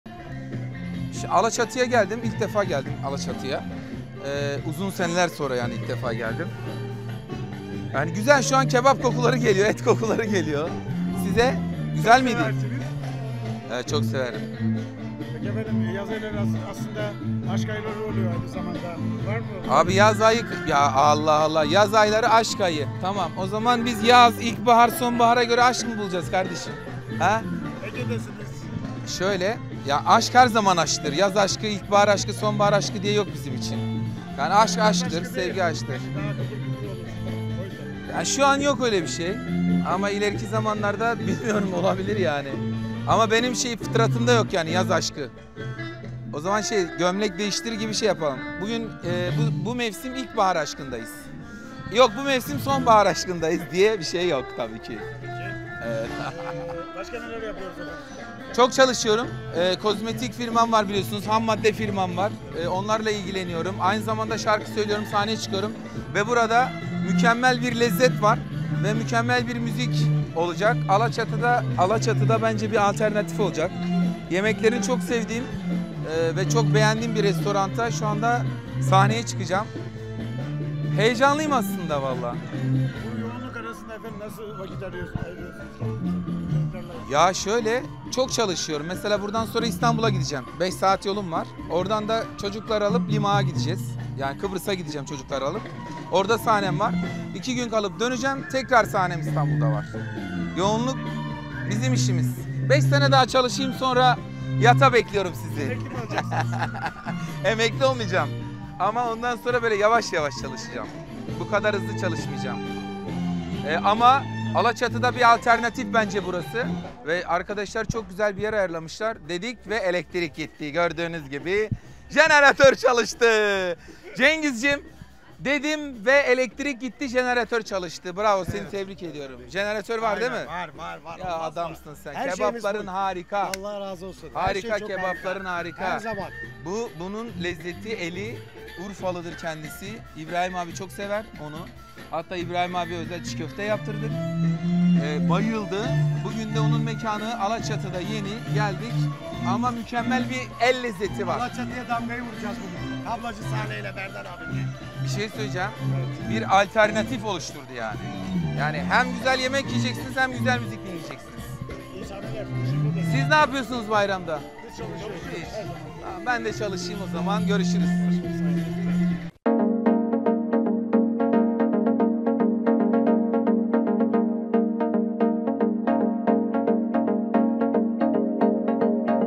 Berdan Mardini'den Mevsimlik Aşk Açıklaması
Uzun zaman sonra Alaçatı'da sahneye çıkan Berdan Mardini magazincilere açıklama yaptı. "YAZ AŞKI FITRATIMDA YOK!"

TÜRKSAT 4A
Frekans: 12729
Sembol: 30000
Polarizasyon: V (Dikey)